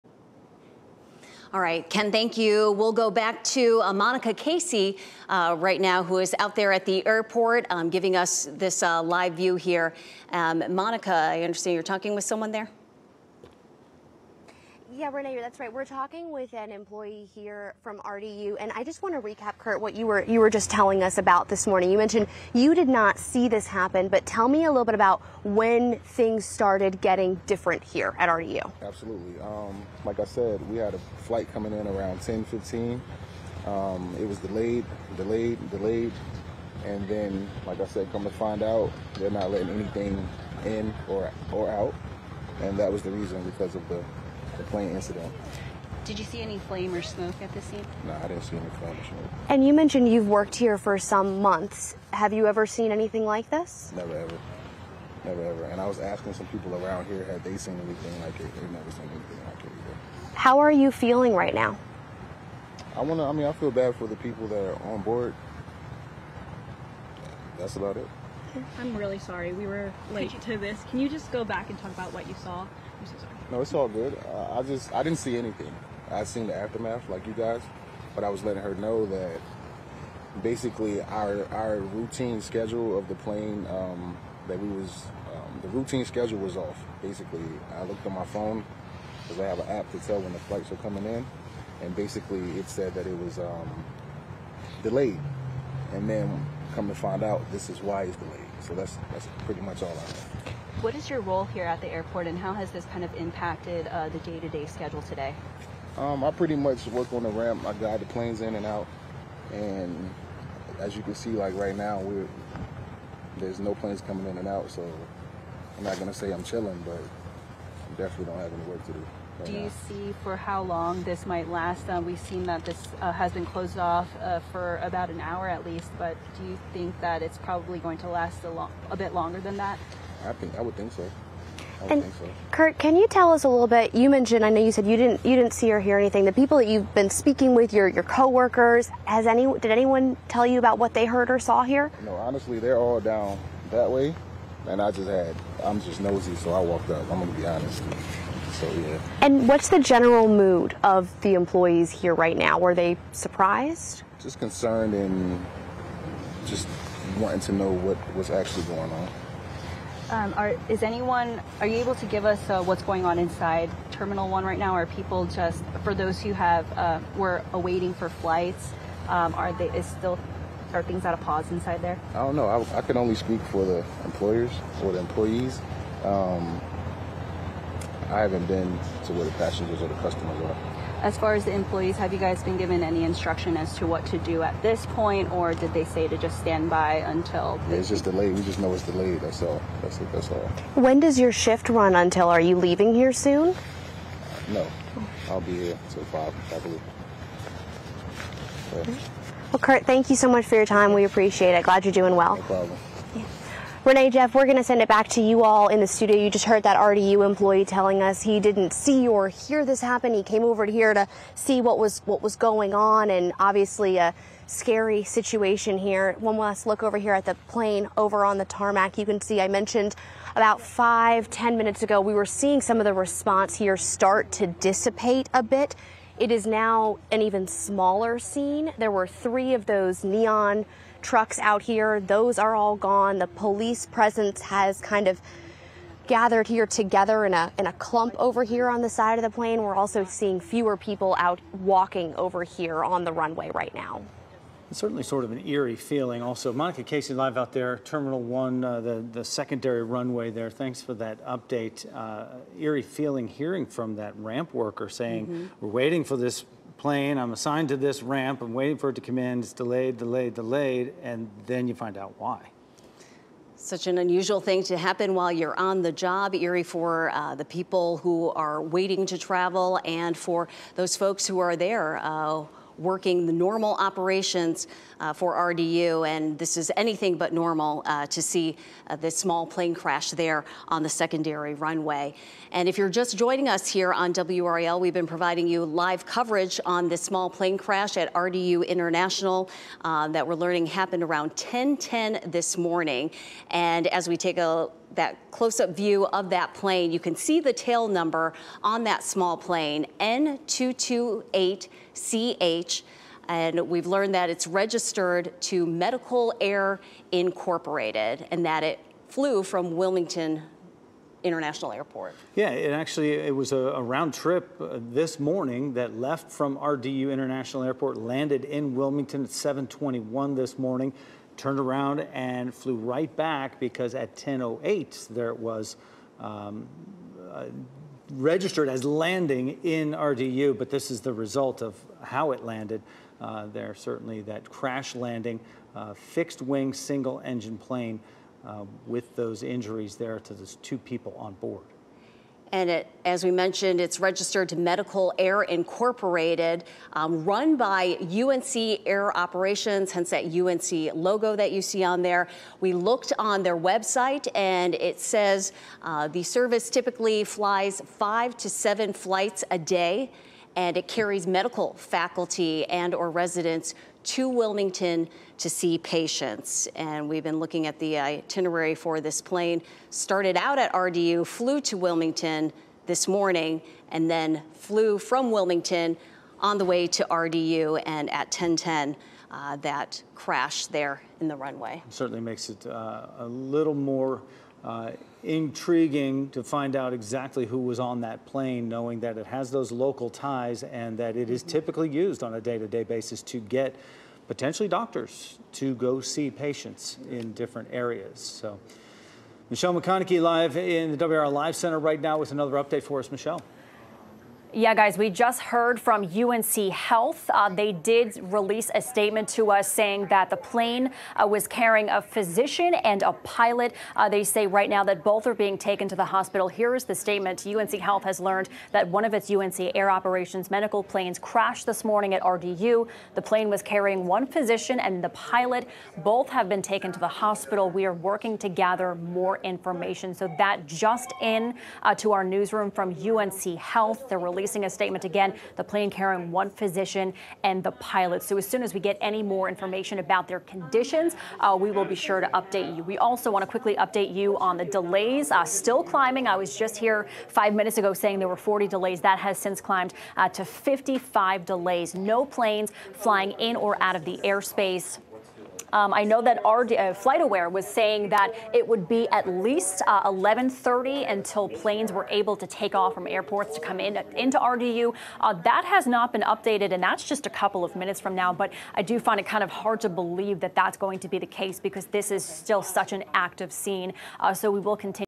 RDU Employee Speaks on Plane Crash: UNC Health Plane crashed at RDU with 1 Physician and a Pilot
According to radio traffic, a small plane crashed at Raleigh-Durham International Airport (RDU) Wednesday morning. It was a UNC Air Operations Medical Planes carrying one physician and the pilot. According to Broadcastify, two people were on board the plane that crashed near gate 15. This happened on a secondary runway at RDU. The airfield is closed for flight operations right now, which means no flights are coming in or out of RDU. All inbound flights to RDU are being held at their origin until 11:30 a.m., according to Flightaware.com.

As of 11 a.m., there are 27 delays at RDU.

WRAL News is working to learn what caused the crash.